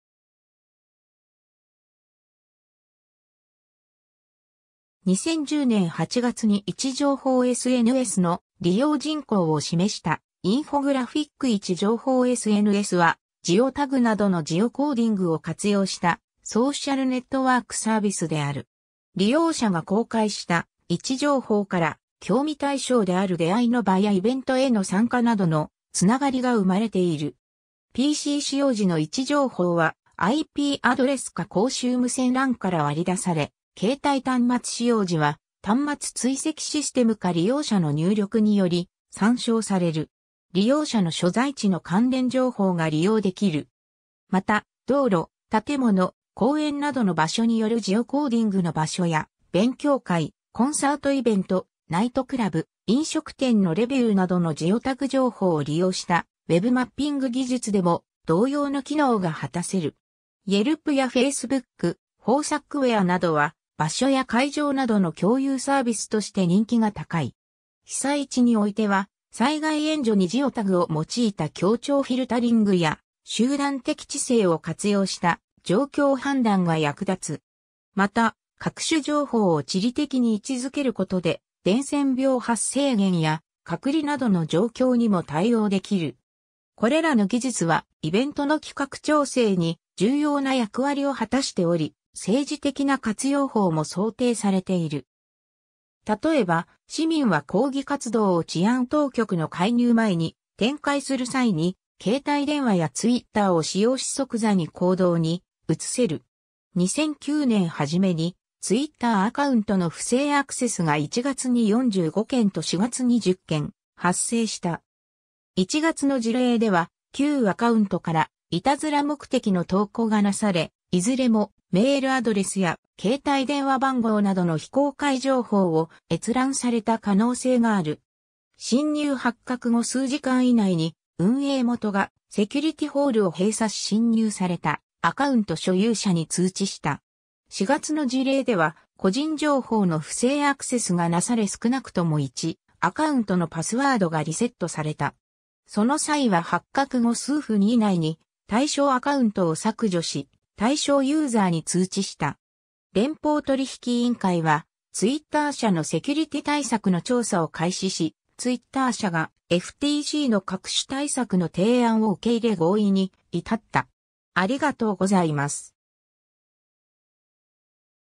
位置情報SNS
ソーシャル・ネットワーキング・サービス
2010年8月に位置情報SNSの利用人口を示したインフォグラフィック 位置情報SNS（いちじょうほうエスエヌエス、英:Geosocial networking service）は、ジオタグなどのジオコーディングを活用したソーシャル・ネットワーク・サービスである。
利用者が公開した位置情報から、興味対象である出会いの場やイベントへの参加などのつながりが生まれている。
PC使用時の位置情報はIPアドレスか公衆無線LANから割り出され、携帯端末使用時は端末追跡システムか利用者の入力により参照される。
 利用者の所在地の関連情報が利用できる。
また、道路、建物、公園などの場所に拠るジオコーディングの場所や、勉強会、コンサートイベント、ナイトクラブ、飲食店のレビューなどのジオタグ情報を利用したWebマッピング技術でも同様の機能が果たせる。
 イェルプやFacebook、Foursquareなどは、場所や会場などの共有サービスとして人気が高い。
 被災地においては、災害援助にジオタグを用いた協調フィルタリングや集団的知性を活用した状況判断が役立つ。
また、各種情報を地理的に位置づけることで伝染病発生源や隔離などの状況にも対応できる。
 これらの技術はイベントの企画調整に重要な役割を果たしており、政治的な活用法も想定されている。
例えば、市民は抗議活動を治安当局の介入前に展開する際に、携帯電話やTwitterを使用し即座に行動に移せる。
 2009年初めに、Twitterアカウントの不正アクセスが1月に45件と4月に10件発生した。
1月の事例では、9アカウントから悪戯目的の投稿がなされ、いずれもメールアドレスや携帯電話番号などの非公開情報を閲覧された可能性がある。
侵入発覚後数時間以内に、運営元がセキュリティホールを閉鎖し侵入されたアカウント所有者に通知した。
4月の事例では、個人情報の不正アクセスがなされ少なくとも1アカウントのパスワードがリセットされた。
その際は発覚後数分以内に対象アカウントを削除し、対象ユーザーに通知した。
連邦取引委員会（以下、FTC）はTwitter社のセキュリティ対策の調査を開始し、Twitter社がFTCの各種対策の提案を受け入れ合意に至った。